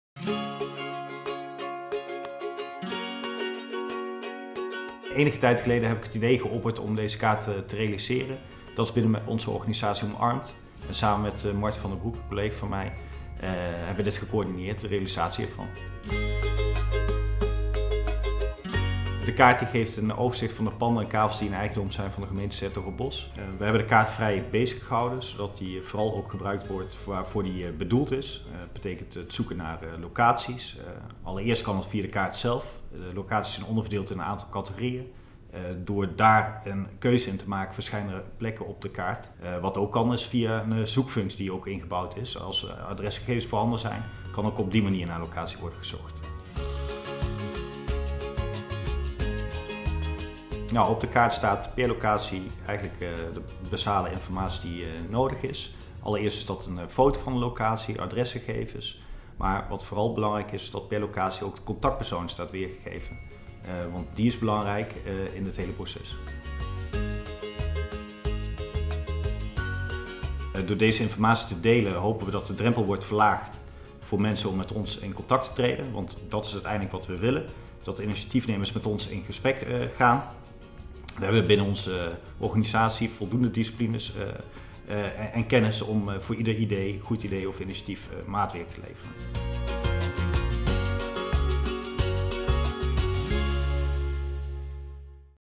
Ontwikkellocatiekaart door Eric Visser
Eric Visser startte binnen 's-Hertogenbosch het initiatief om een digitale ontwikkellocatiekaart te starten. Benieuwd wat dit precies is? Eric legt het in het kort zelf even uit!